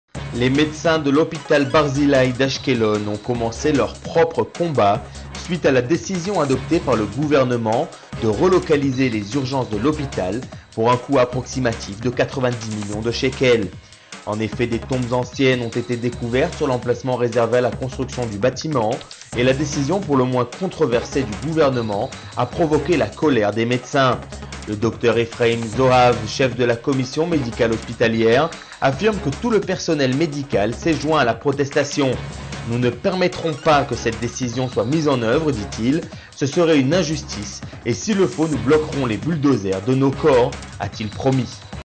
Barzilai: nous bloquerons les bulldozers de nos corps !
Les médecins de l'hôpital Barzilaï d'Ashkelon ont commencé leur propre combat suite à la décision adoptée par le gouvernement de relocaliser les urgences de l'hôpital pour un coût approximatif de 90 millions de shequels. En effet, des tombes anciennes ont été découvertes sur l'emplacement réservé à la construction du bâtiment, et la décision pour le moins controversée du gouvernement a provoqué la colère des médecins. Le Dr. Efraim Zohav, chef de la commission médicale hospitalière, affirme que tout le personnel médical s'est joint à la protestation: "Nous ne permettrons pas que cette décision soit mise en œuvre", dit-il. "Ce serait une injustice et s'il le faut nous bloquerons les bulldozers de nos corps!" a-t-il promis.